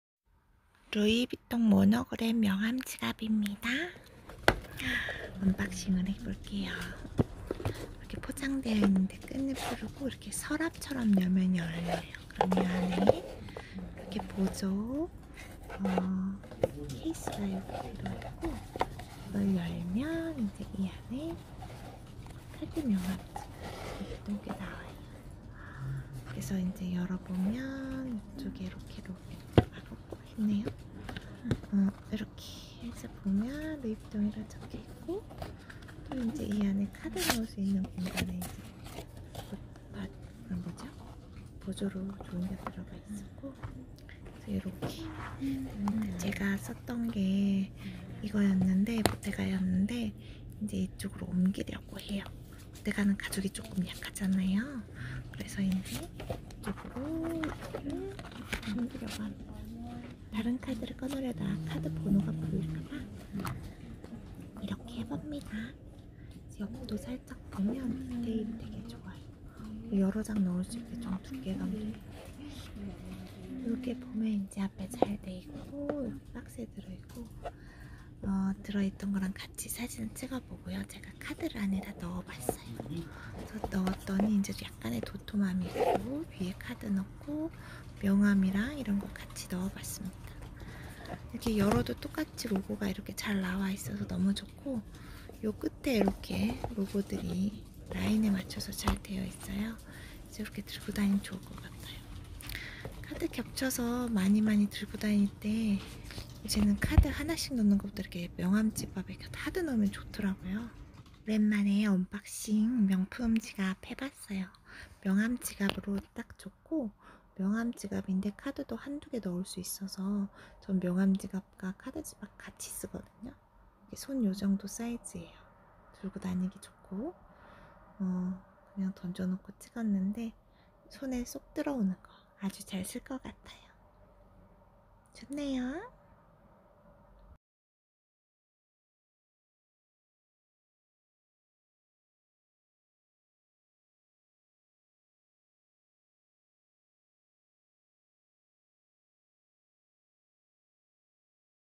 루이비통 모노그램 Louis Vuitton monogram 명함지갑, 카드지갑... 튼튼해서 좋은! 언박싱 리뷰
언박싱 리뷰 루이비통 모노그램 명함지갑, 카드지갑... 튼튼해서 좋은!
Louis Vuitton monogram

명함지갑을 카드지갑으로 쓰면 좋다.
카드지갑을 명함지갑으로 써도 좋다.
그런 좋은 지갑...
나의 짱가가 준 지갑!
보테가베네타 카드지갑은 잠시 쉬고 이제 루이비통 카드지갑으로!!!